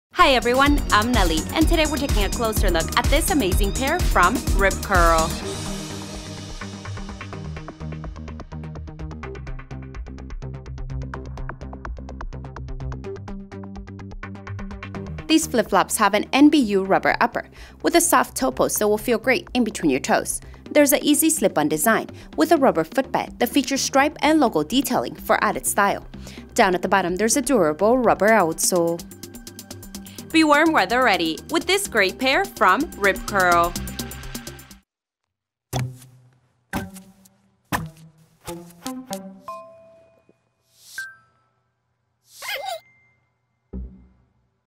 Rip Curl Shibori Stripe SKU:8756716
Keep your style solid for the sand with the Rip Curl® Shibori Stripe sandal!
NBU rubber upper.
Embossed logo detailing adorns strap.
Rubber footbed with stripe and logo detailing.
Rubber outsole for durable wear.
Imported.